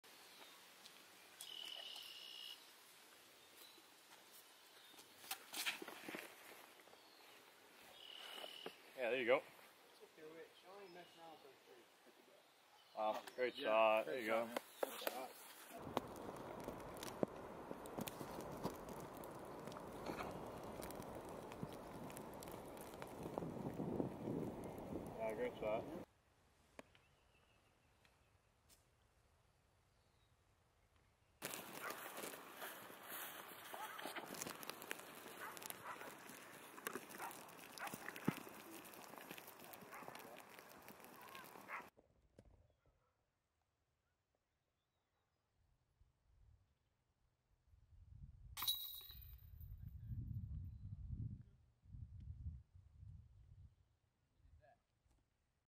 Nikko Locastro’s brother Jeremiah CRUSHES 💪
Jeremiah Dwyer, Nikko Locastro's brother, traveled from his home in St. Louis to Kentucky for the Lexington Open. He was able make lead card for round 2 and stay there for the final round, along the way, there were some highlights.